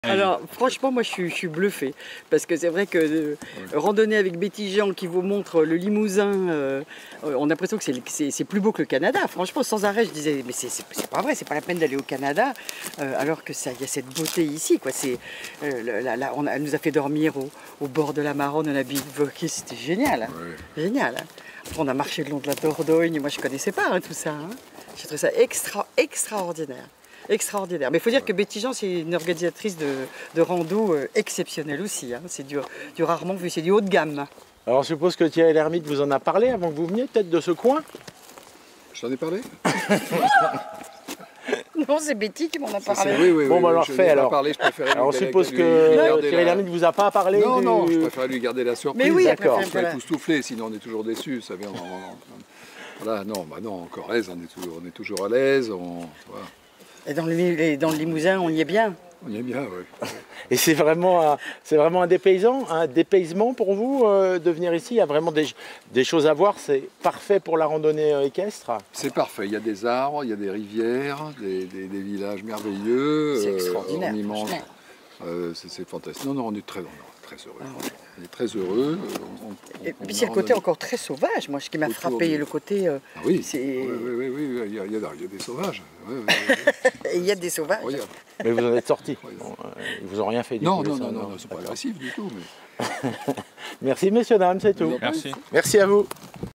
Charlotte de Turckheim et Thierry Lhermitte en Corrèze
Les deux comédiens participent à une randonnée équestre en Xaintrie.
L'un est un habitué, l’autre découvre. Les deux comédiens sont d'accord sur un point : pas besoin d'aller à l'autre bout du monde pour être dépaysé.

Entretien : Thierry Girault et Laurent du Rusquec